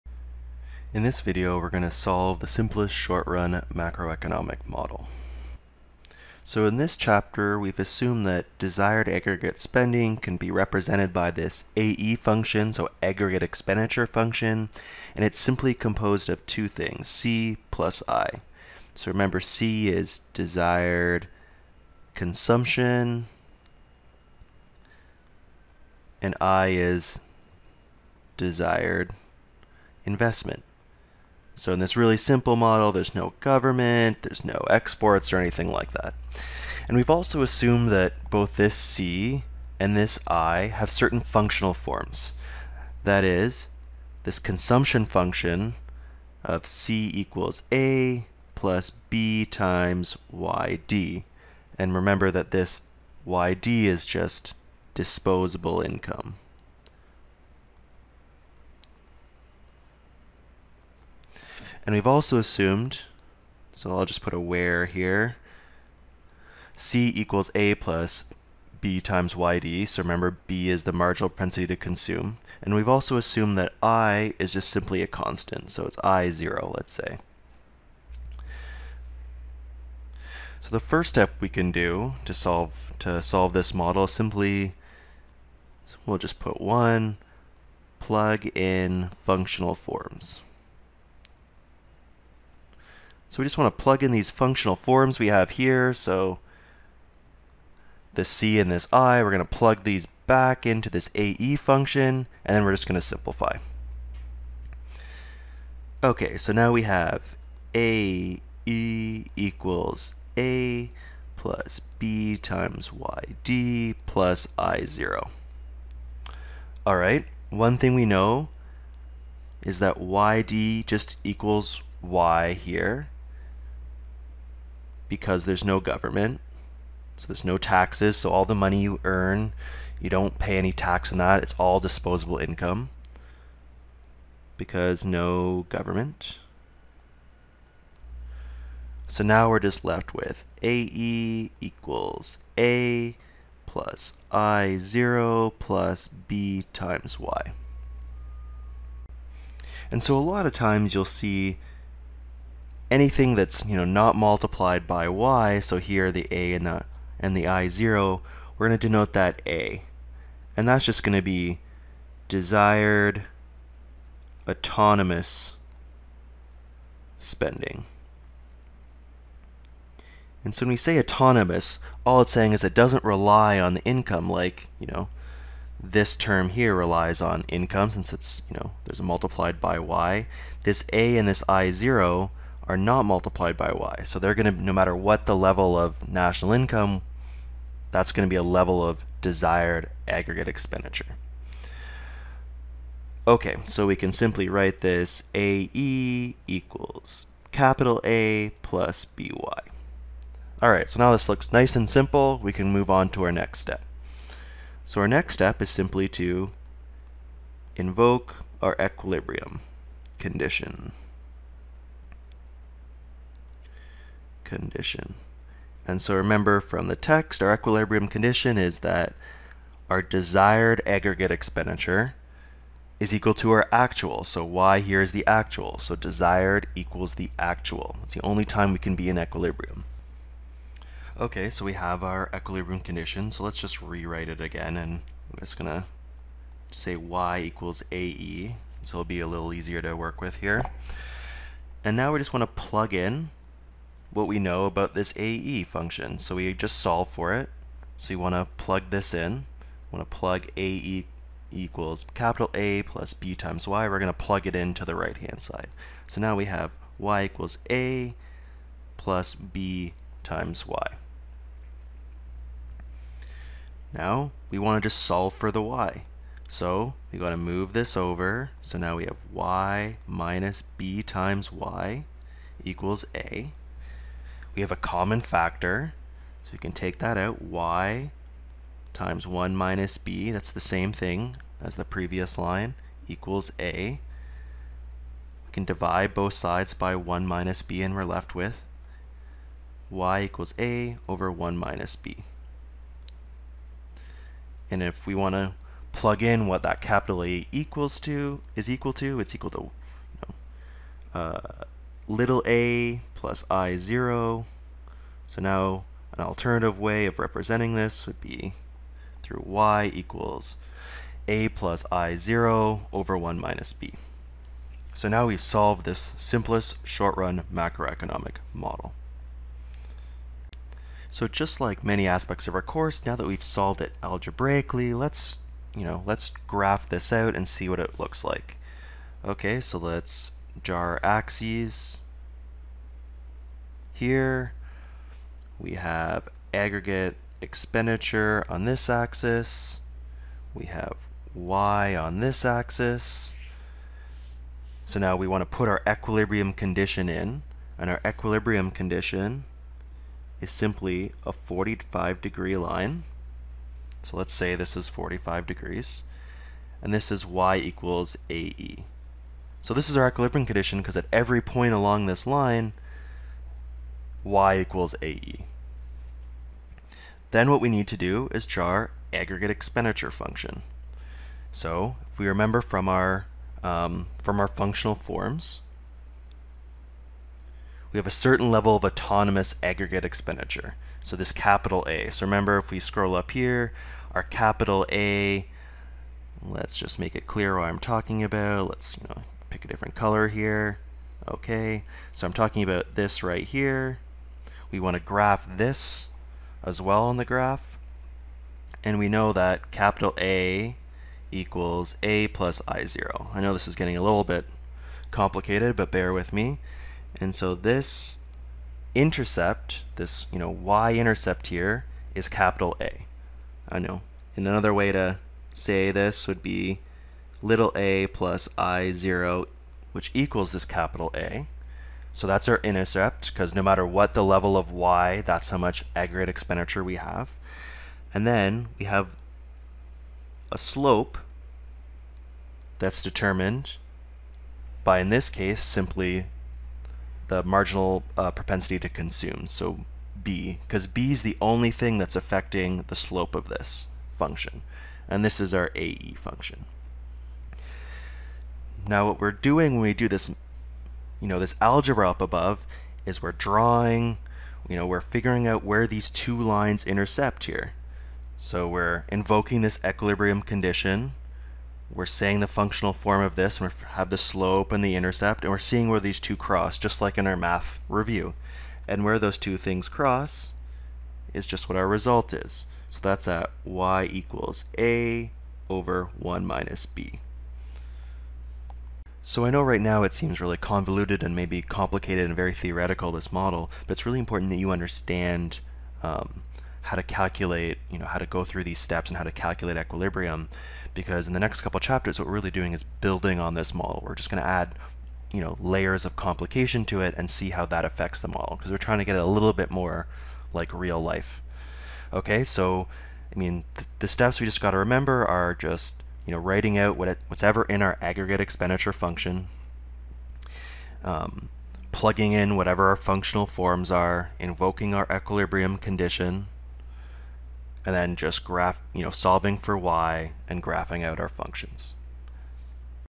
Ragan - Chapter 21- Simplest Short-run Model - Algebra
In this video we will review solving the simplest short-run model from Ragan (chapter 21)